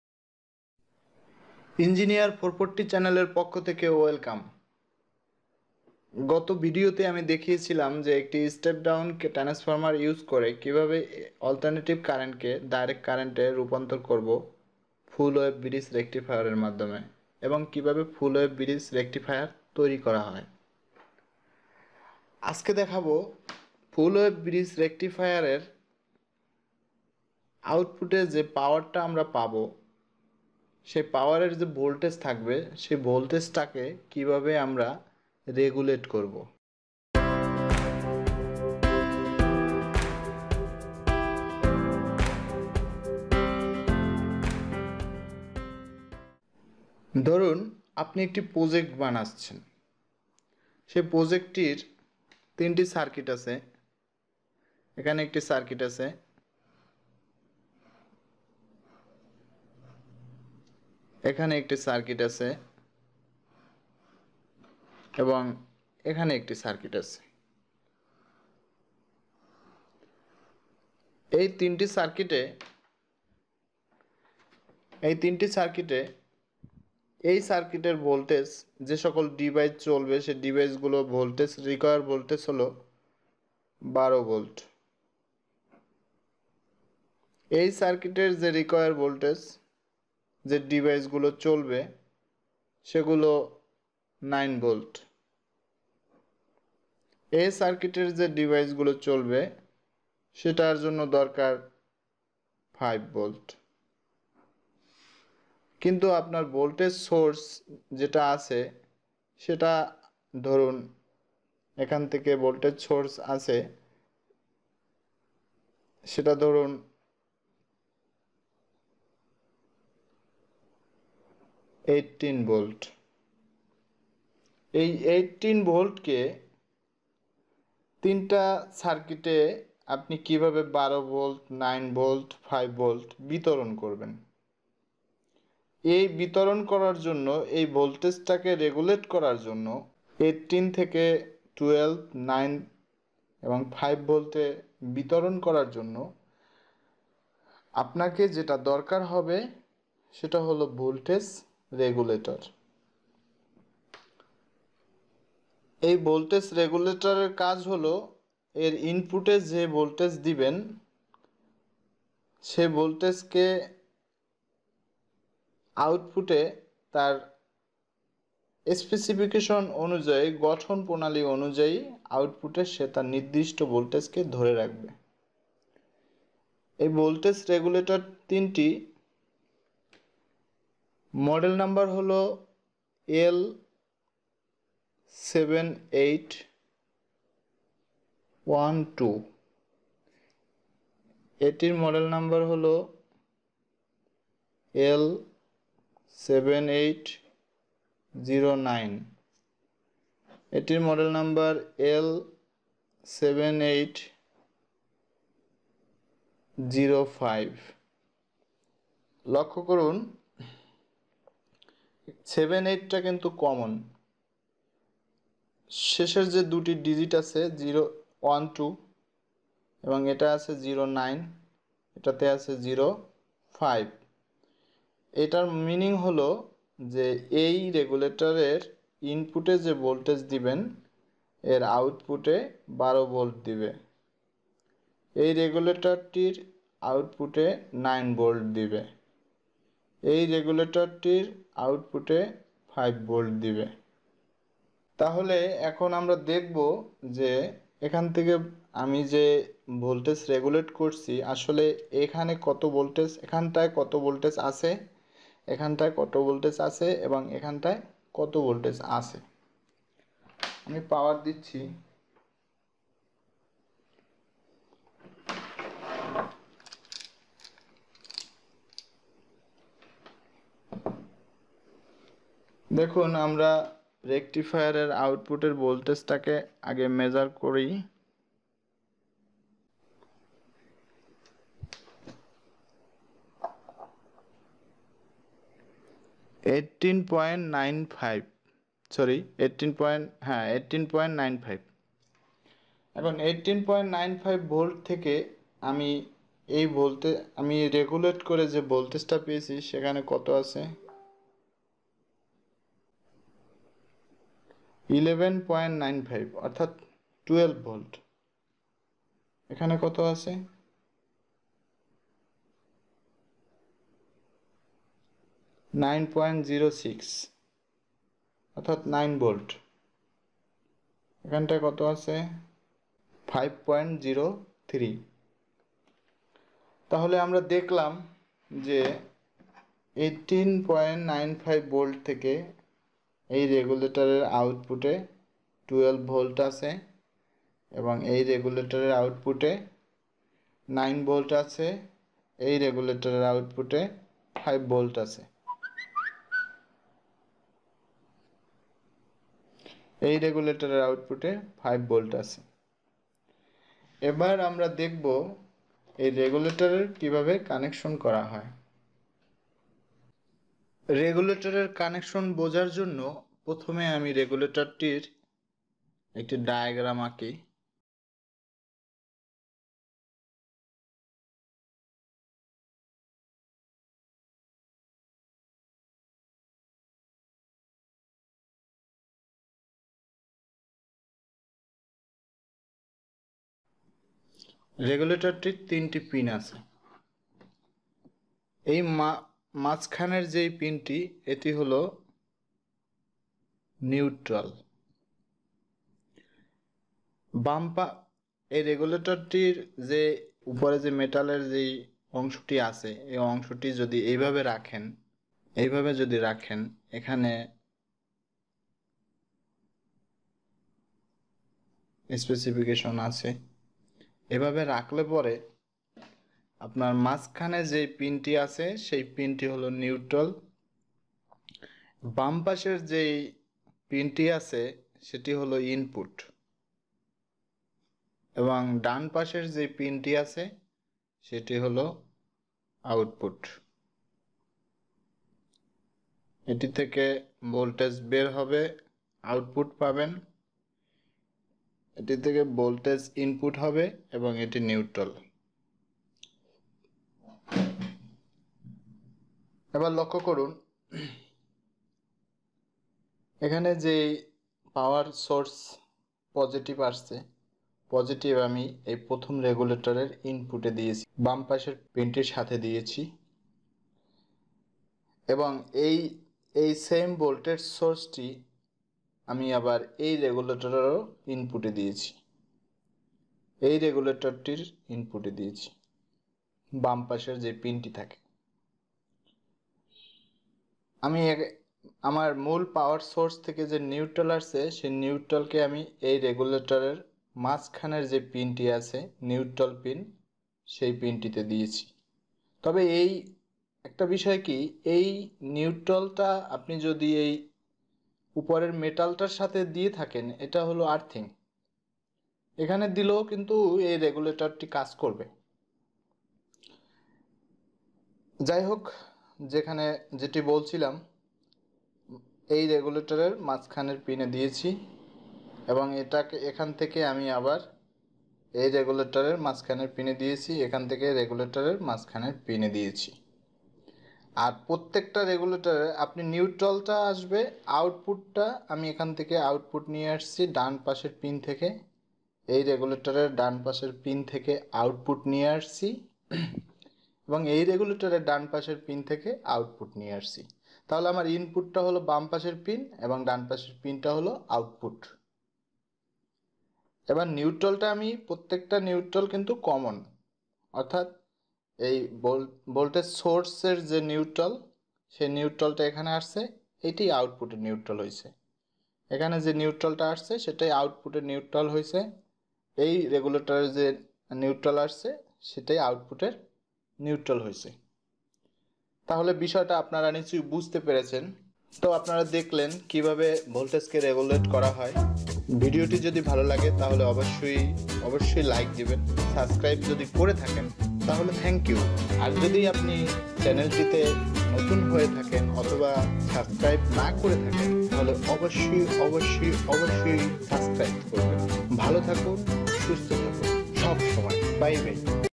voltage regulator | in bangla
this video provided to learn about how to make voltage regulator for individual circuit from one power source. you can get knowledge on voltage regulator. connection of voltage regulator.